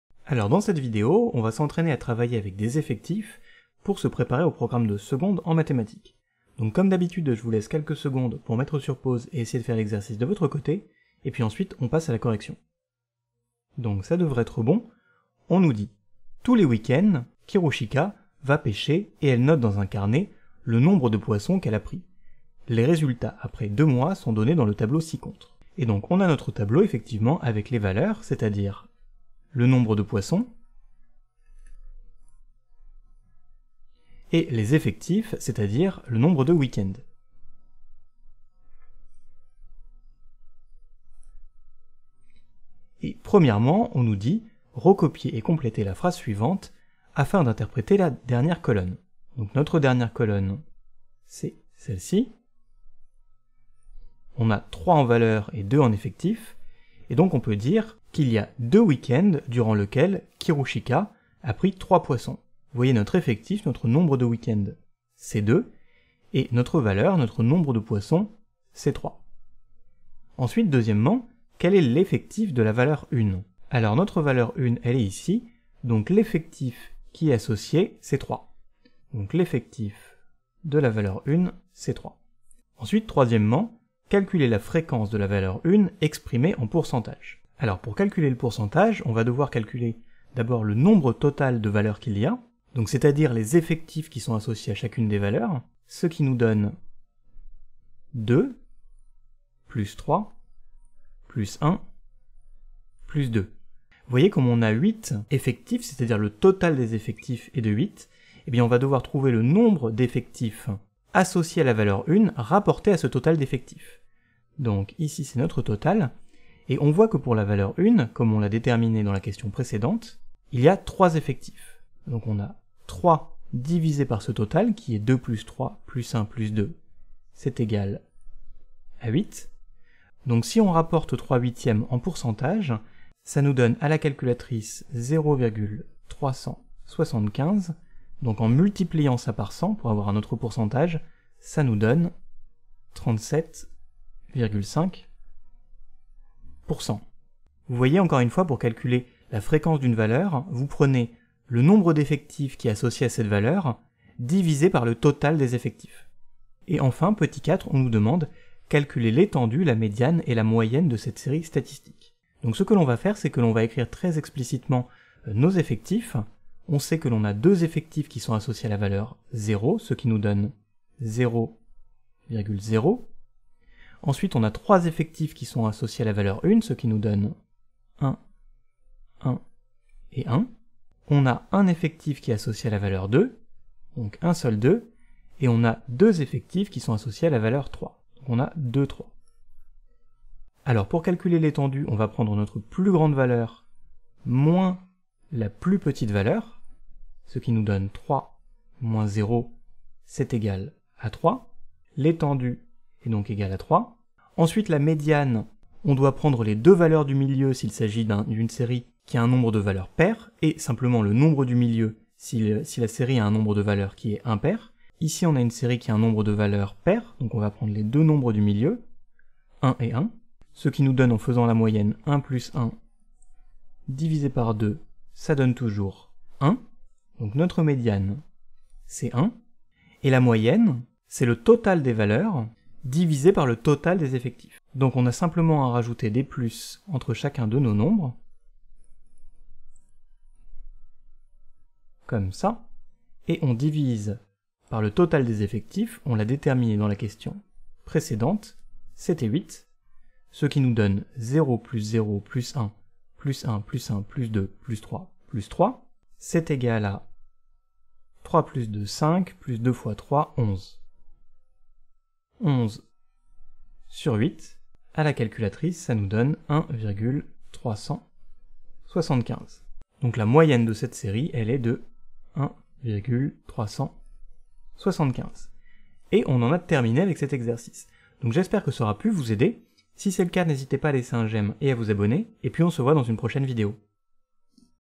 Mathématiques - Se préparer aux statistiques descriptives (2)
Retrouvez-moi en live sur Twitch Dans cette vidéo nous allons nous entraîner à travailler avec des effectifs afin de préparer le programme de seconde en mathématiques.

Table des matières:
0:00 - Question 1.
1:10 - Question 2.
1:30 - Question 3.
2:30 - Question 4.

Sources:
Manuel Magnard Sésamath Seconde